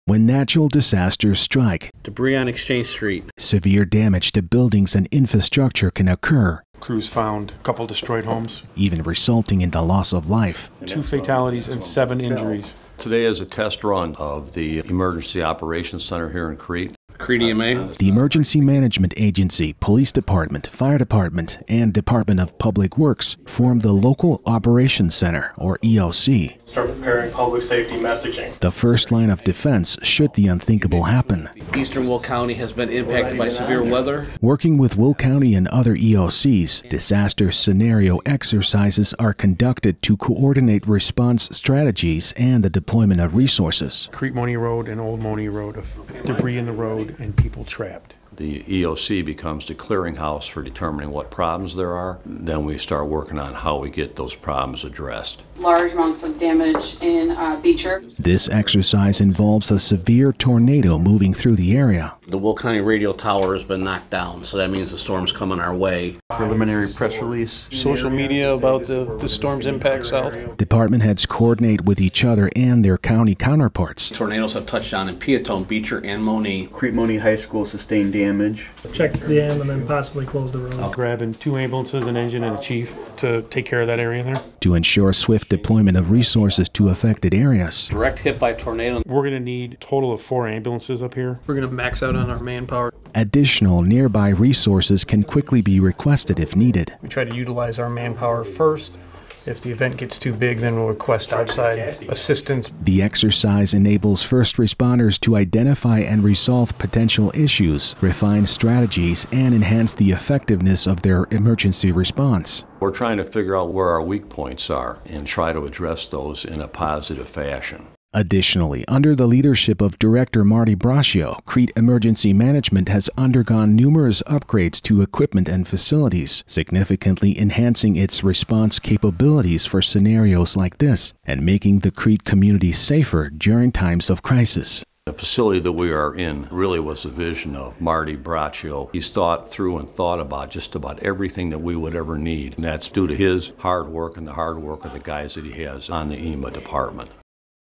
Crete Tornado Preparedness
80% of Illinois tornadoes occur during the late PM hours between April 1st and June 30th. Last year, Village of Crete department heads participated in a county-wide exercise to enhance community preparedness for such events. Participants included EMA Director Marty Braccio, Crete Police Chief Scott Pieritz, the late Fire Chief James JP Muirhead, and Department of Public Works Superintendent Justin Pancrazio. The exercise yielded valuable data, which helped optimize emergency response plans.